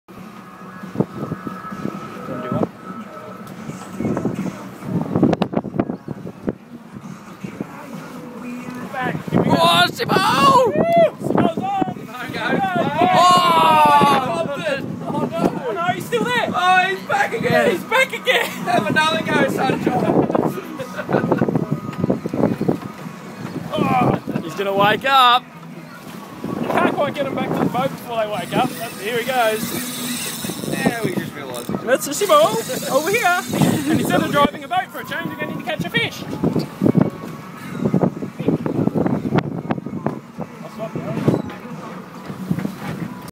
Red hot tuna fishing charters
Skipper Simon rinaldi catching blue fin tuna on soft plastics